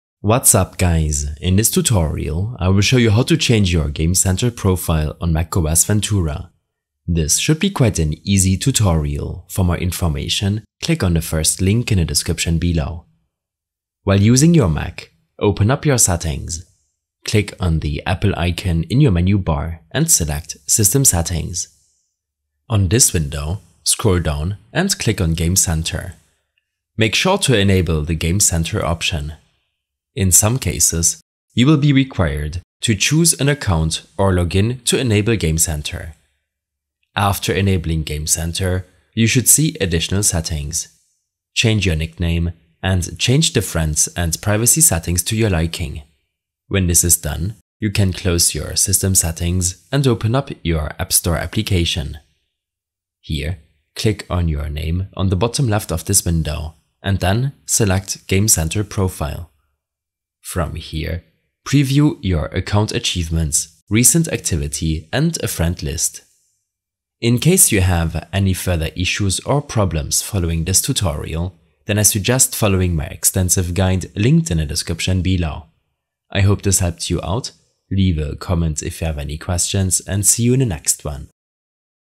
Mac OS Ventura - How To Change Game Center Profile
A short tutorial on how to change your game center profile in Mac OS Ventura.

Timestamps:
0:00 Introduction
0:14 Enable Center Settings
0:37 Game Center Settings
0:47 Game Center Profile